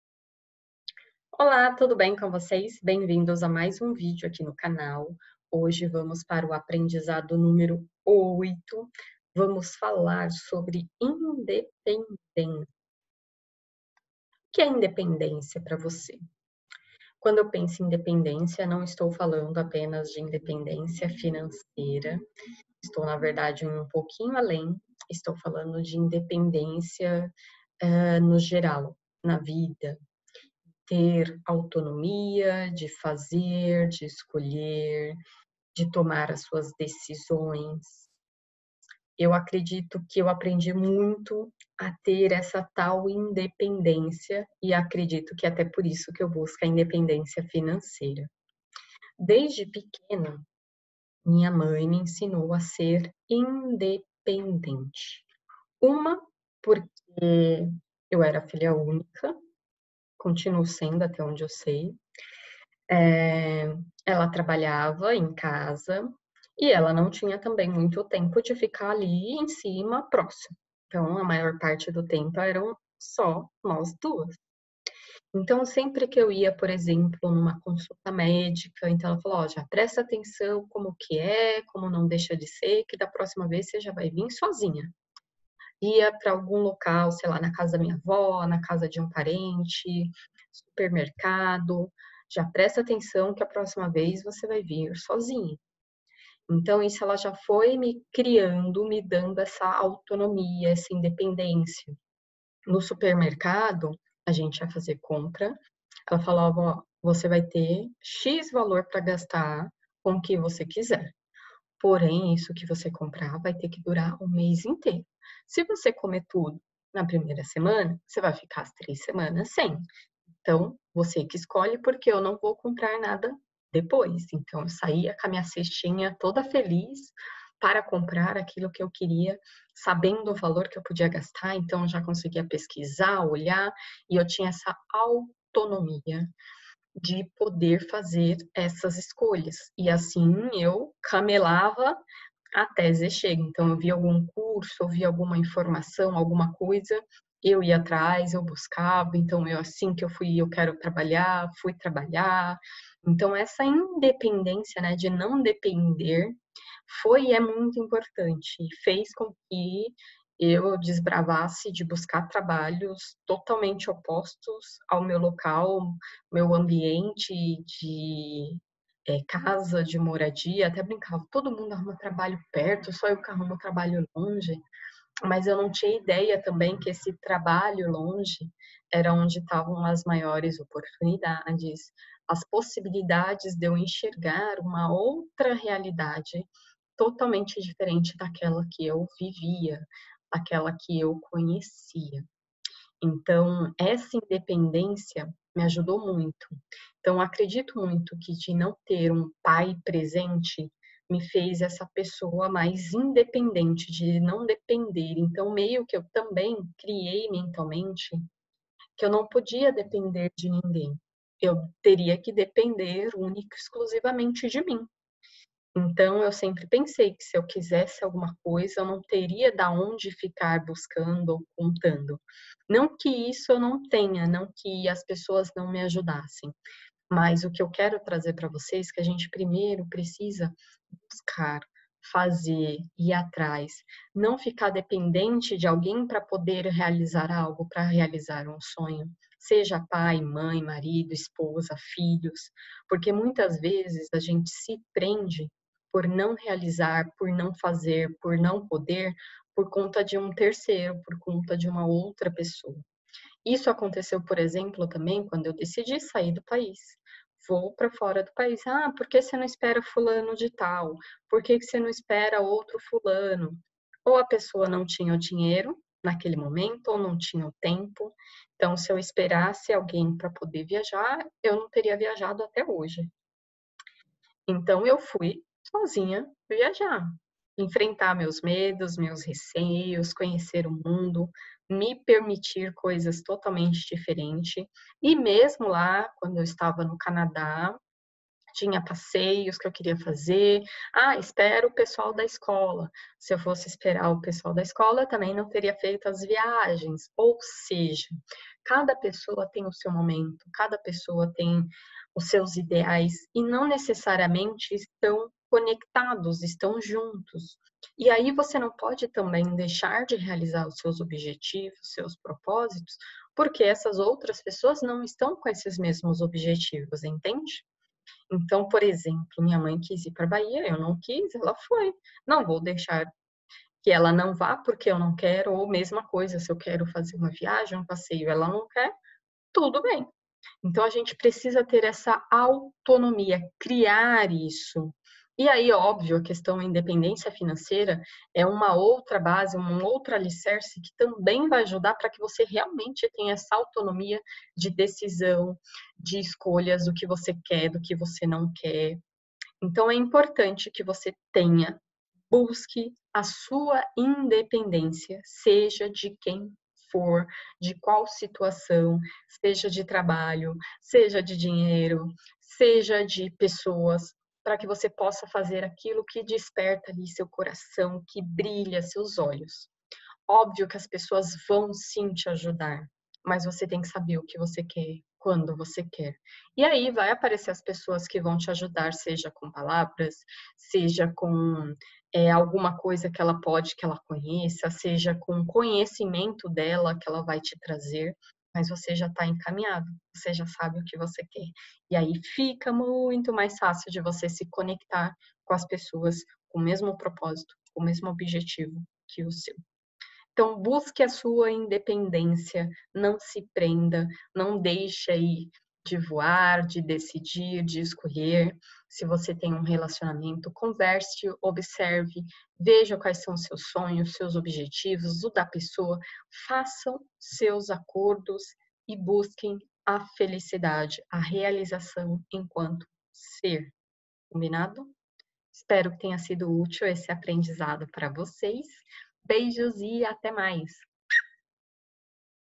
#08 - Independência | 33 Aprendizados
Olá,
O oitavo aprendizado é sobre Independência.

Espero que esse compartilhar, de alguma forma possa lhe ser útil. Deixe nos comentários quais aprendizados obteve com o tema INDEPENDÊNCIA!

Lembre-se, somos eternos aprendizes!
Abraços,